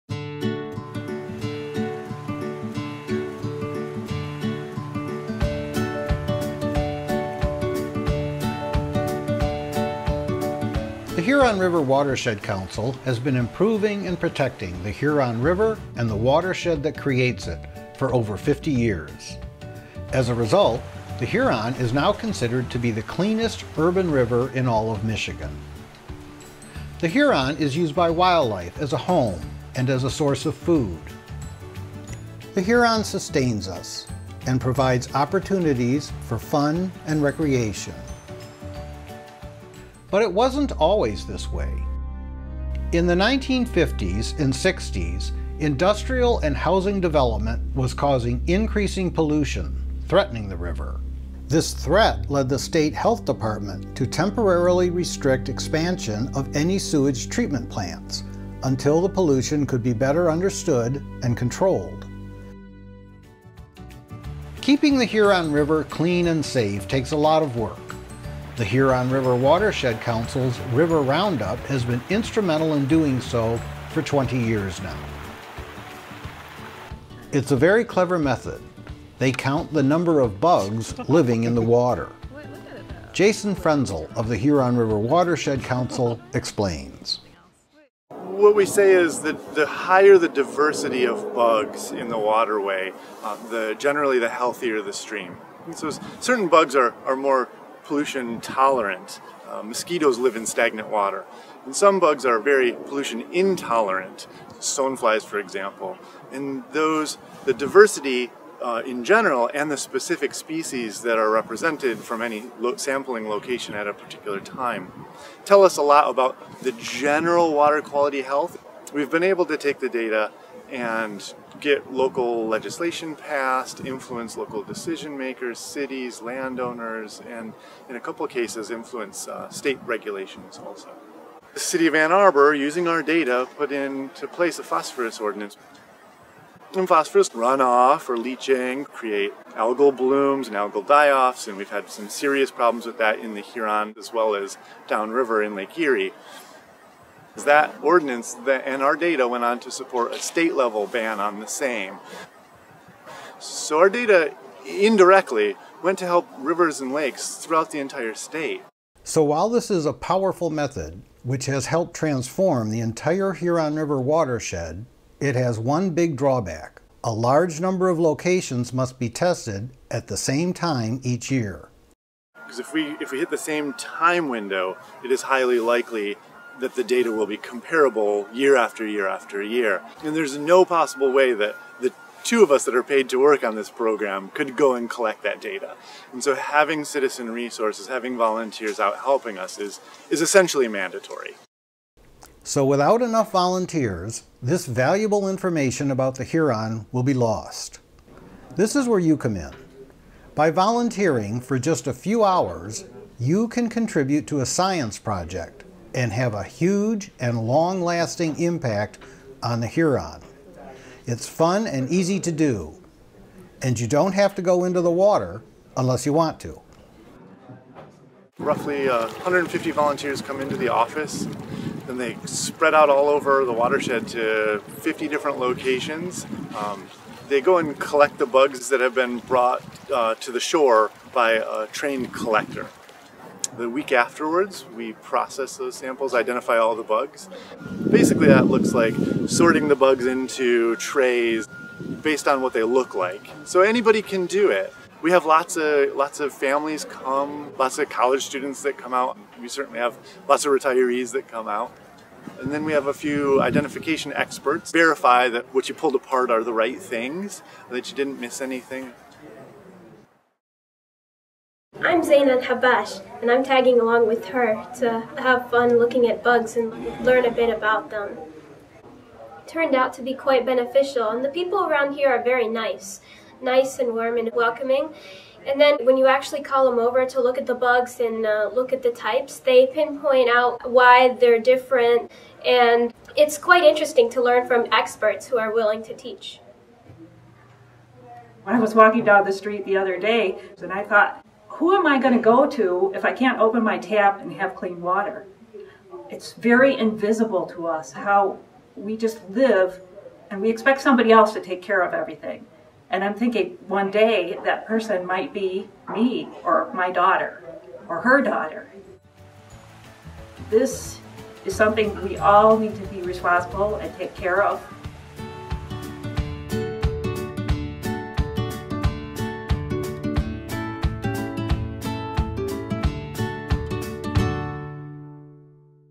HRWC River Roundup
At HRWC's River Roundup, a long-running study of the Huron River, volunteers get to know a special place in the Huron's tributaries, while helping us learn about the river's health. At this all-age friendly event, trained volunteers guide small teams in fieldwork to search through river samples for pollution-sensitive 'bugs' that live in our waterways. The event is followed by Insect ID Day where additional volunteers become scientists for a day, identifying the species found.
Volunteer videographer: David Brown

David produced "HRWC River Roundup" from concept to creation and did all the scripting, narrating, directing, filming, music and editing. Thank you David for your support of HRWC and the Huron River!
With Jason Frenzel, Zaina Al Habash and Laurie Domaleski